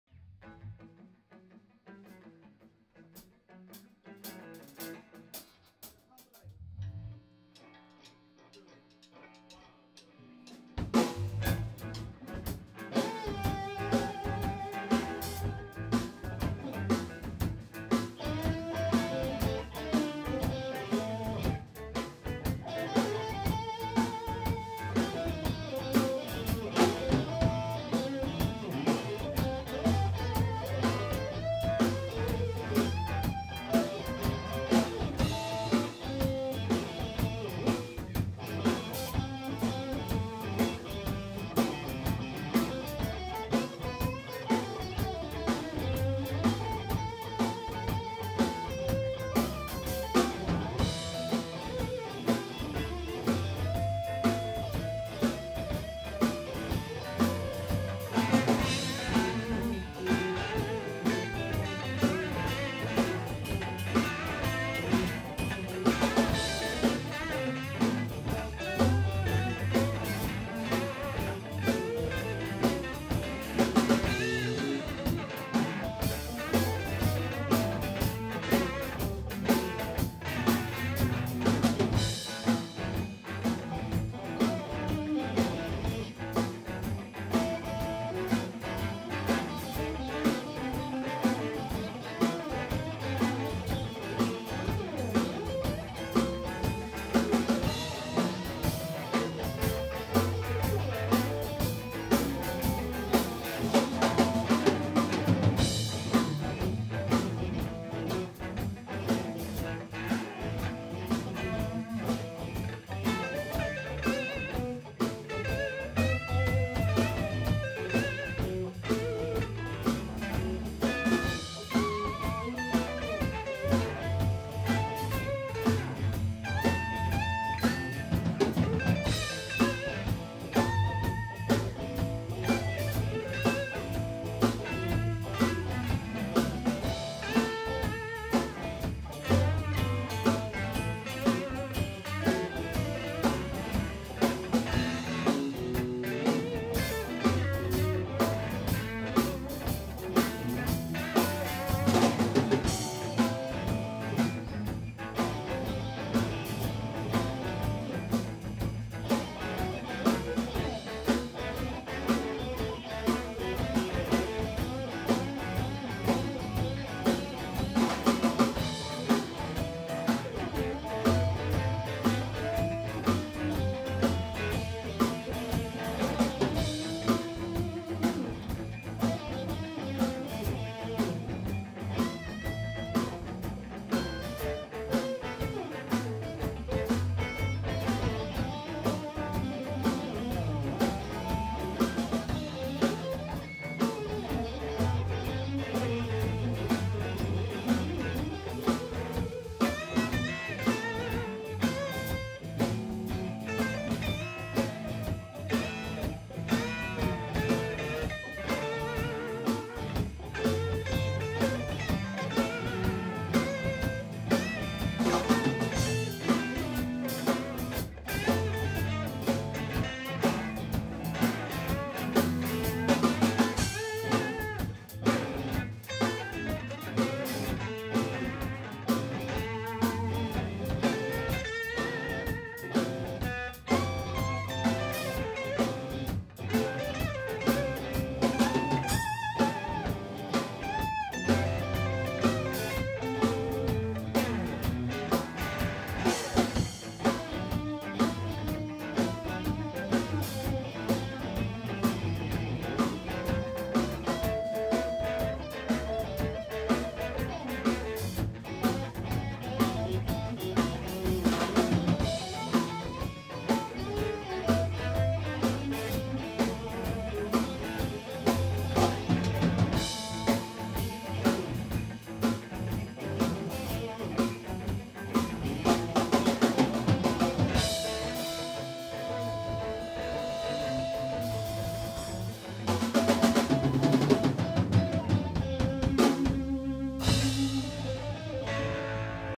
Friday Night Jam20150313 KeyAブルース
2015年3/13に島村楽器伊丹昆陽店で開催されました「Friday Night Jam」の模様です。